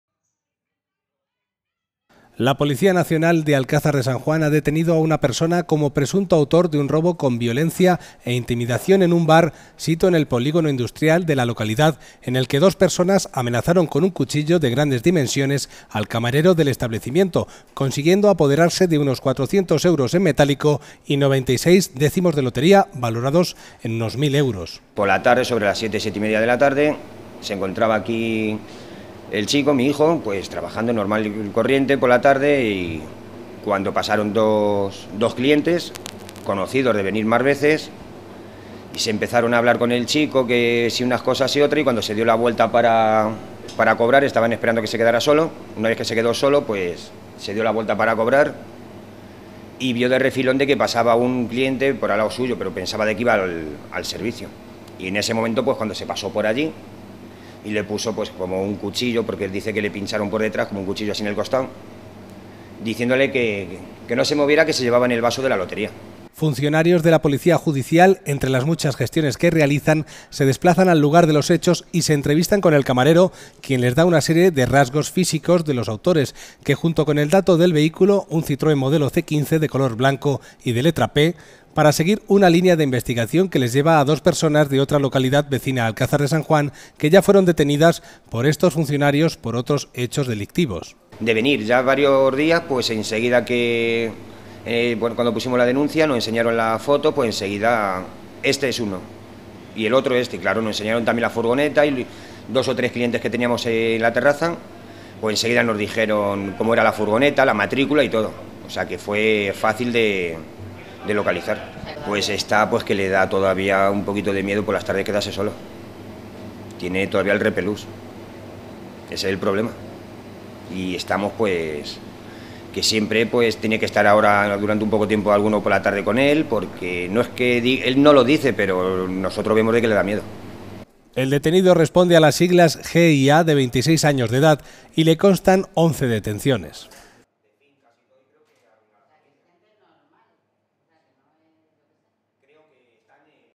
Atraco en Cafe Bar Javi
La Policía Nacional de Alcázar de San Juan ha detenido a una persona como presunto autor de un robo con violencia e intimidación en un bar sito en el polígono industrial de la localidad en el que dos personas amenazaron con un cuchillo de grandes dimensiones al camarero del establecimiento consiguiendo apoderarse de unos 400 euros en metálico y 96 décimos de lotería valorados en unos 1000 euros.